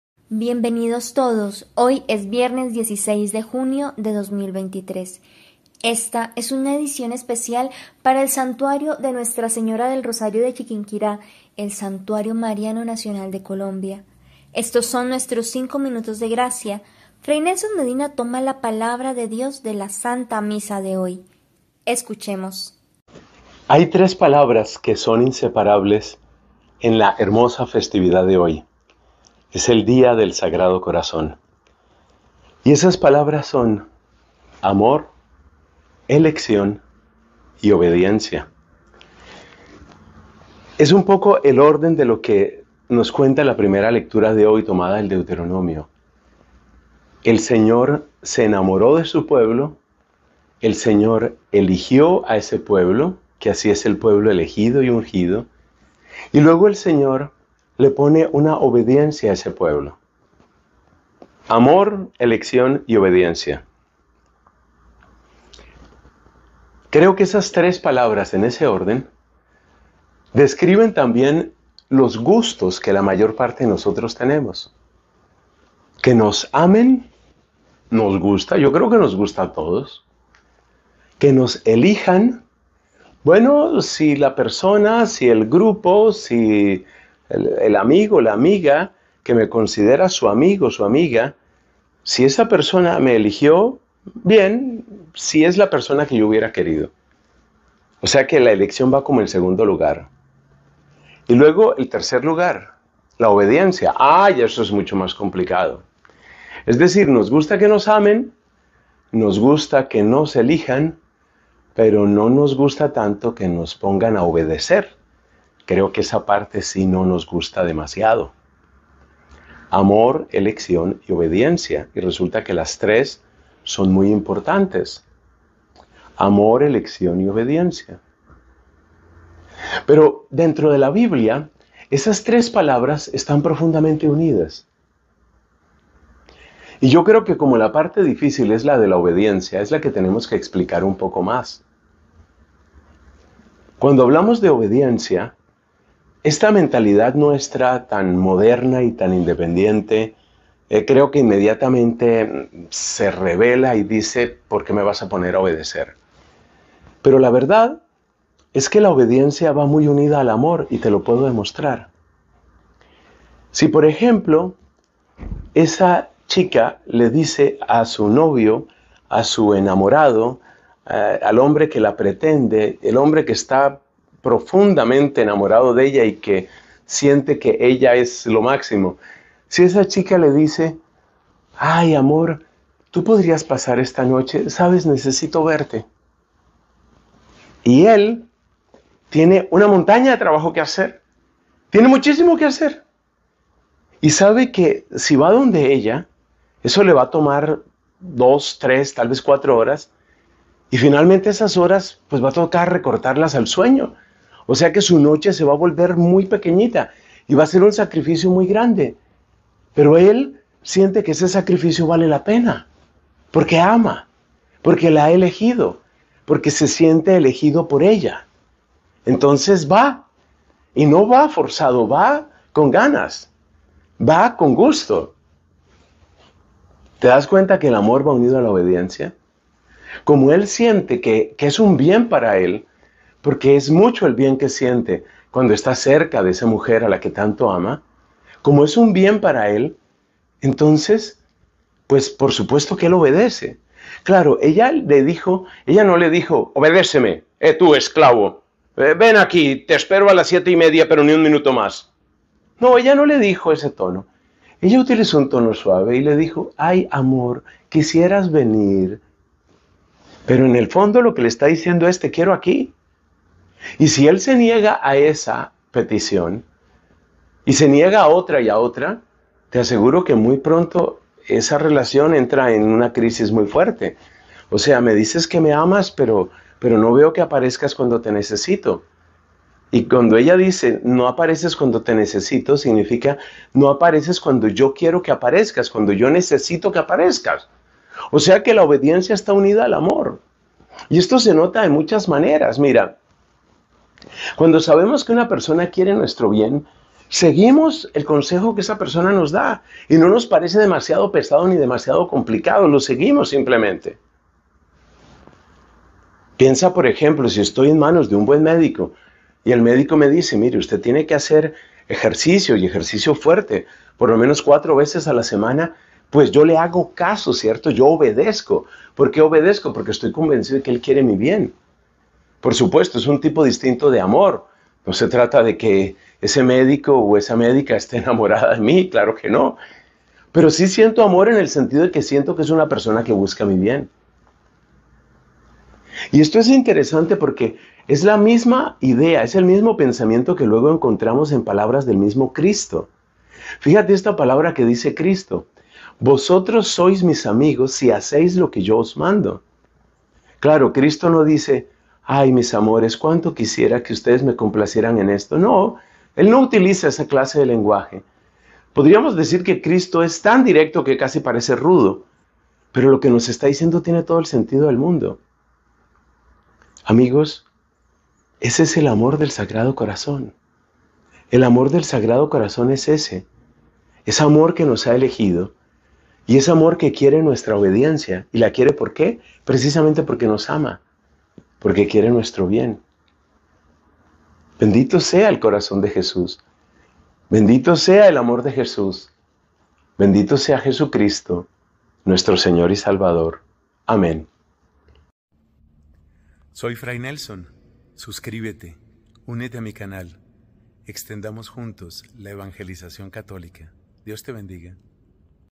¡LA GRACIA! Viernes, 16 de Junio de 2023
SOLEMNIDAD DEL SAGRADO CORAZÓN DE JESÚS - El amor del Sagrado Corazón es amor que nos ha elegido, que quiere nuestra obediencia y la quiere precisamente porque nos ama, porque quiere nuestro bien. * * *